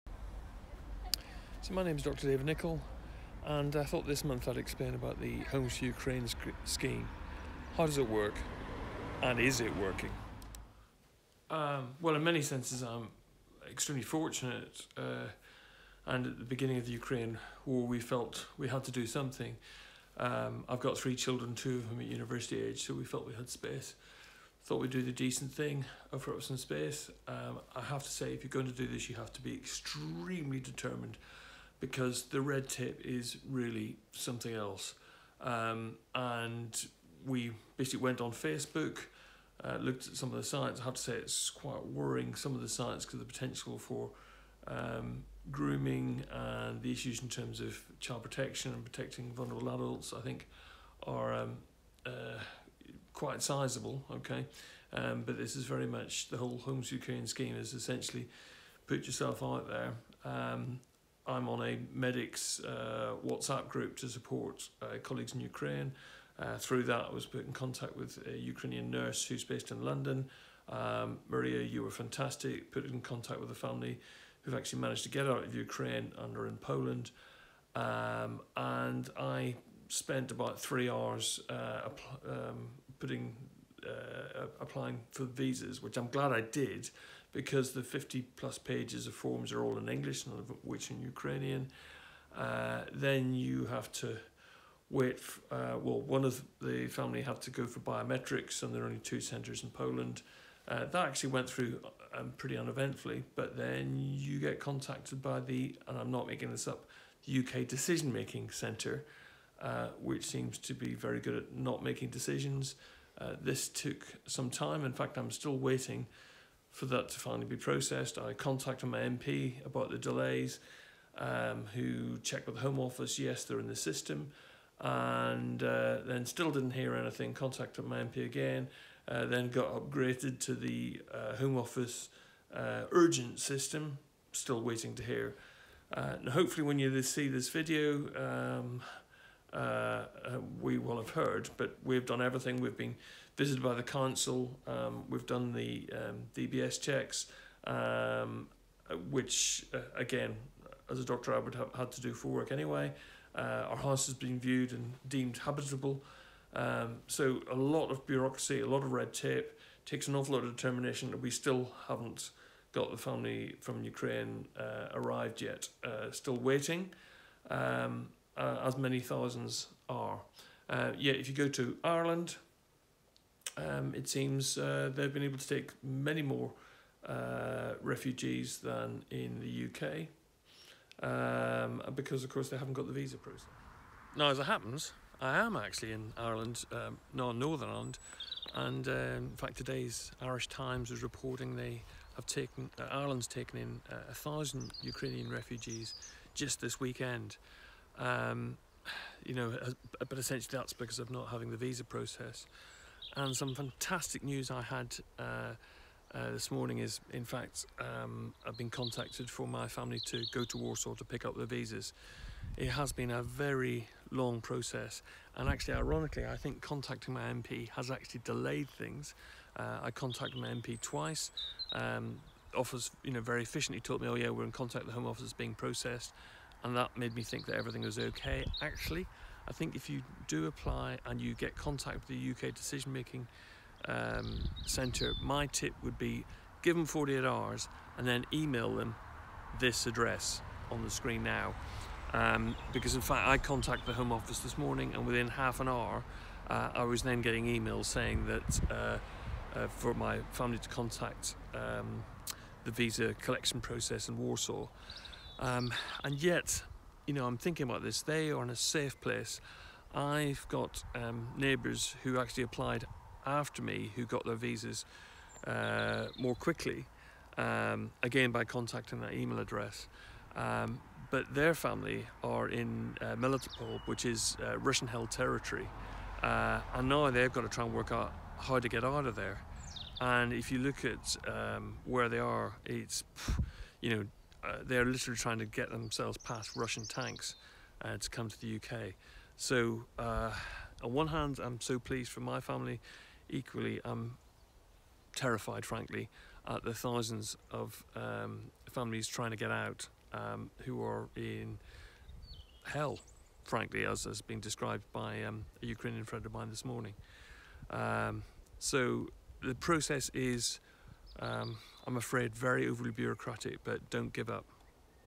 Homes for Ukraine be persistent!!
My top tips on how to manage you're way around the Homes for Ukraine scheme. Be VERY persistent but dont contact your MP, especially if a government MP, it may delay things- better of contacting the Home Office directly AFTER you have heard from the UK decision-making centre